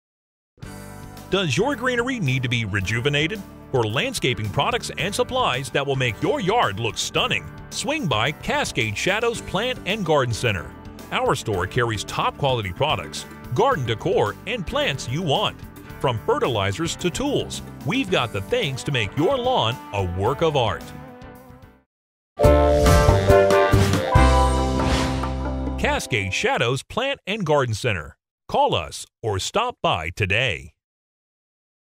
Cascade Shadows Plant & Garden Center
Does your greenery need to be rejuvenated? For landscaping
products and supplies that will make your yard look
stunning, swing by Cascade Shadows Plant and Garden